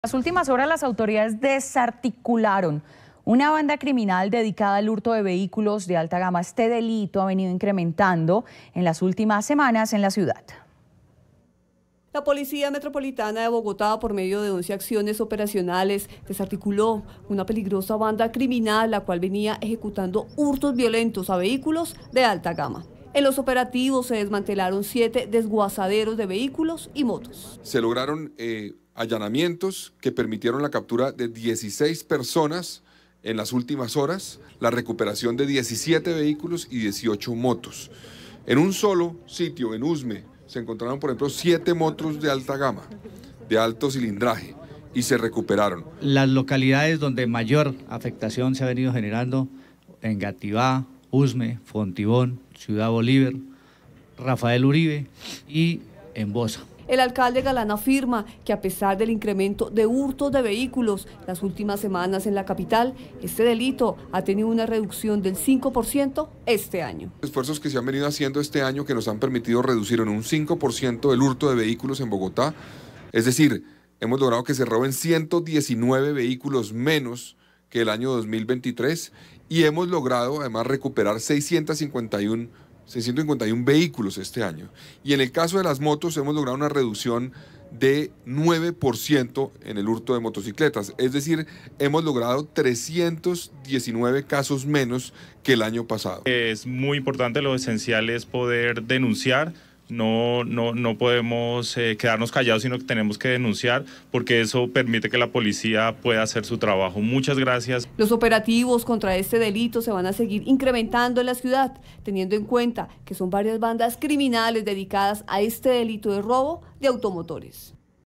Así fue desarticulada una banda dedicada al hurto de vehículos de alta gama en Bogotá
En los operativos fueron desmantelados siete deshuesaderos dedicados a desmantelar carros y motos robadas.